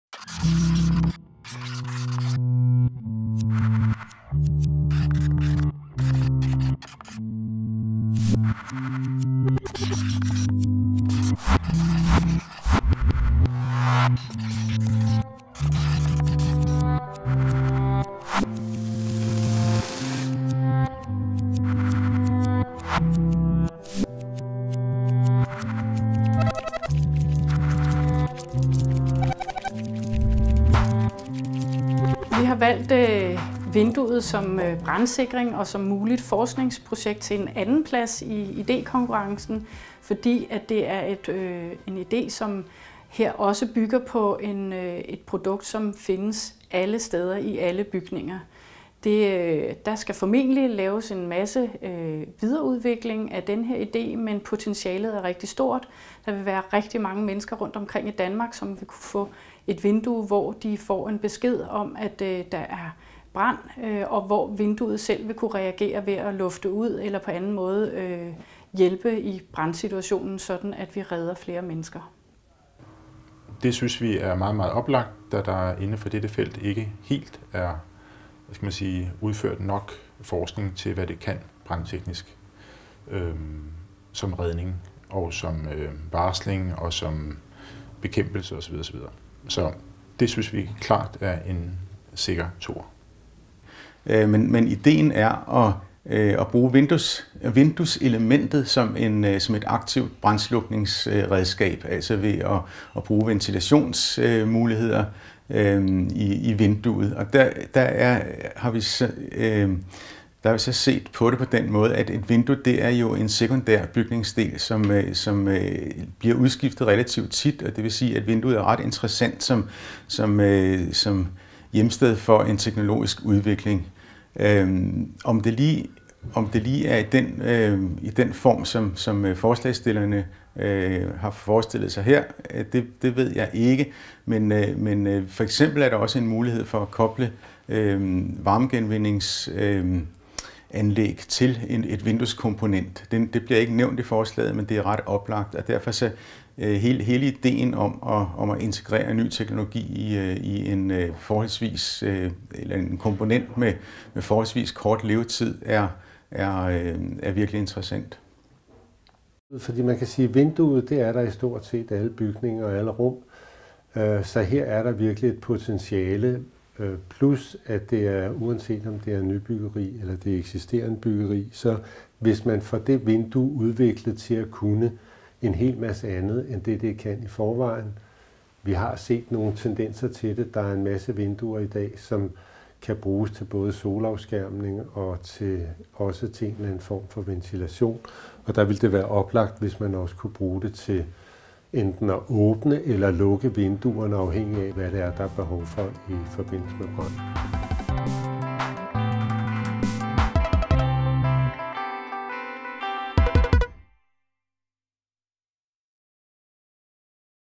Fire Away - 2. prize
We have chosen "The Window as Fire Insurance" for 2nd place in the idea competition because this, too, is an idea that builds on a product that is in all buildings everywhere. This idea will probably have to go through a lot of continued development, but the potential is huge, and there will be a lot of people all over Denmark who will be able to get a window that will tell them that there is a fire, and where the window itself will react by ventilating or otherwise assisting with fire fighting efforts, enabling us to rescue more people.

We find this highly relevant; in this field, there has not been enough research in terms of its capabilities with regard to fires such as warning or rescuing people, fire fighting, etc. For this reason, we think it is a clear 2nd place.

The idea is to use the window element as an active fire fighting tool by utilizing its ventilation capabilities. After all, a window is a secondary building component that is replaced relatively often, making windows an interesting basis for technological development. I don't know whether this is exactly what the proposal is aimed at. Another option is to connect the heat recovery system to a window component. The proposal does not mention this, but it is quite relevant, and this is what makes integrating new technology in a component with a relatively short lifetime a really interesting idea.

Windows are to be found in virtually all buildings and rooms, so this has great potential, regardless of whether it is a construction project or an existing building. If we get windows with a lot of new features, as we have seen in the past with sunscreens and ventilation, it will also be relevant to be able to either open or close windows, depending on what is needed during a fire.